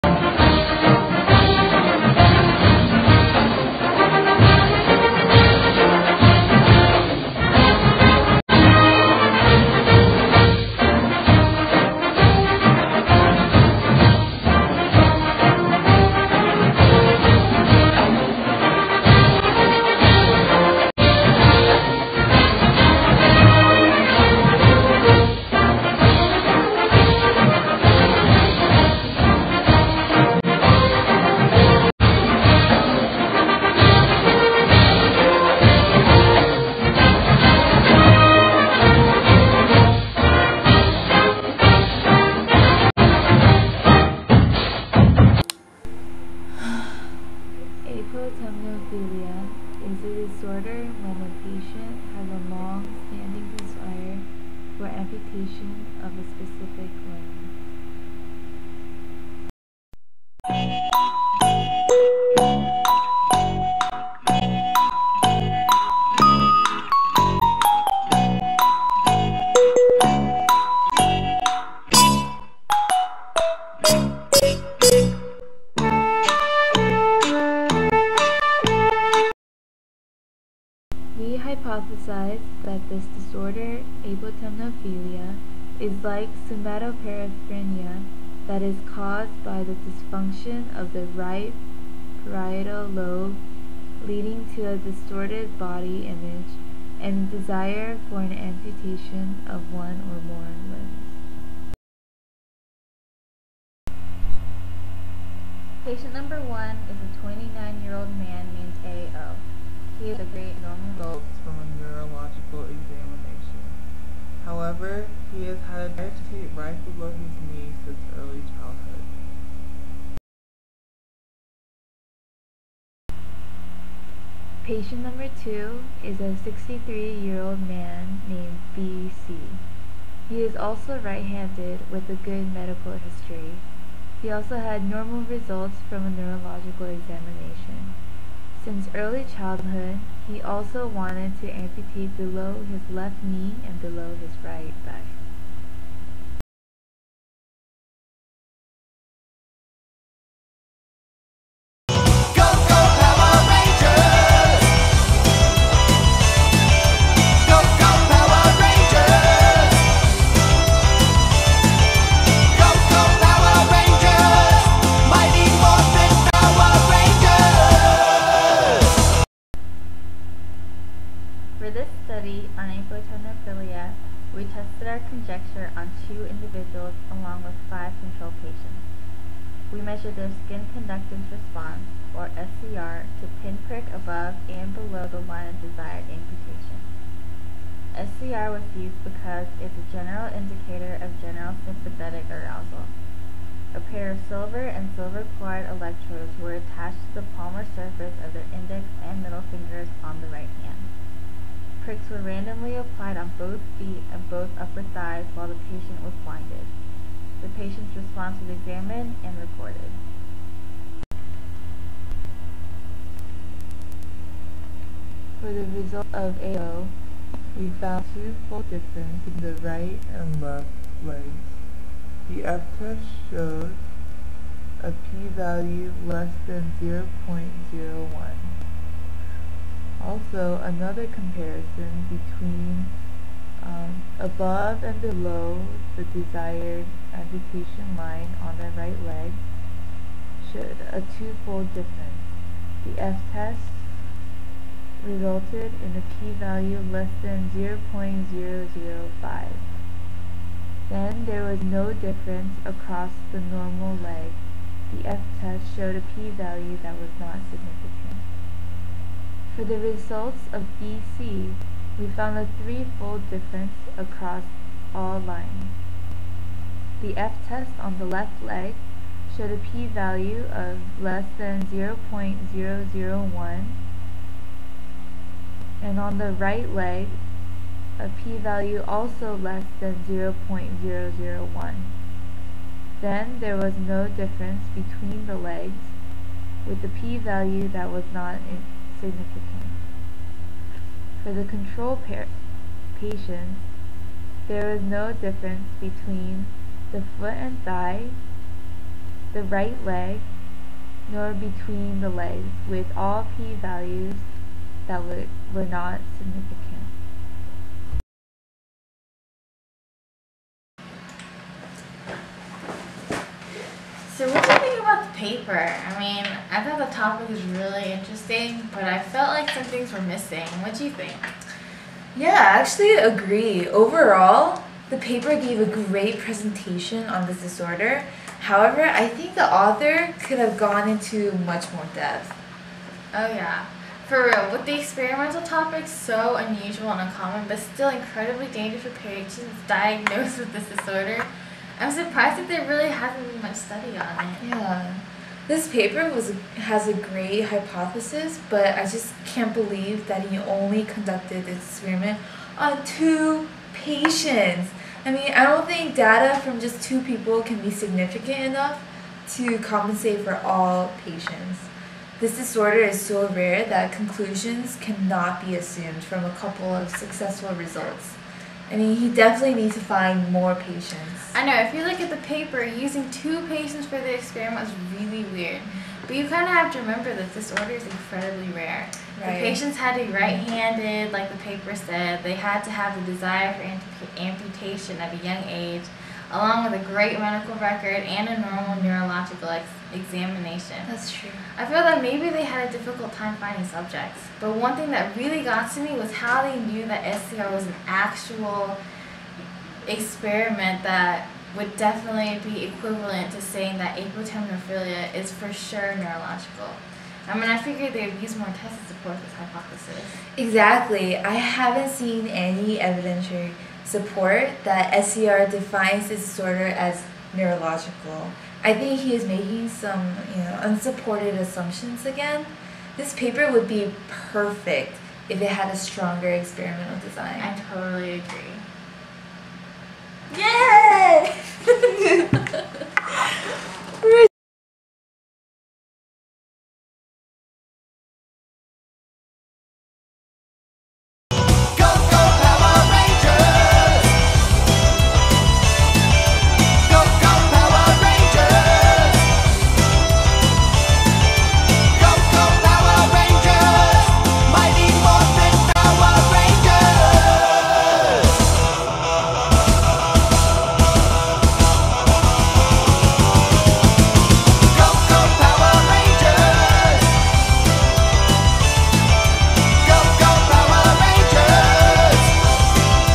Apotemnophilia: a neurological disorder - REVIEW
Georgia Tech
BIOL/BMED 4752
Professor: Dr. Steve Potter
Students: Jenny Kim and Sonia Im

This was a class assignment. We had to do a review of a scientific article of our choice pertaining to a neurological disorder we are studying. The one I am particularly studying is Apotemnophilia  - the self desired amputee. We reenacted the paper and did a critical review.

Paper:
Apotemnophilia: a neurological disorder
David Brang, Paul D. McGeoch and Vilayanur S. Ramachandran

We had a lot of fun filming this video and we hope you enjoy watching it too!

[we do realize that there are 6 power rangers. it was a mistake on our part - we got too excited and didn't double check]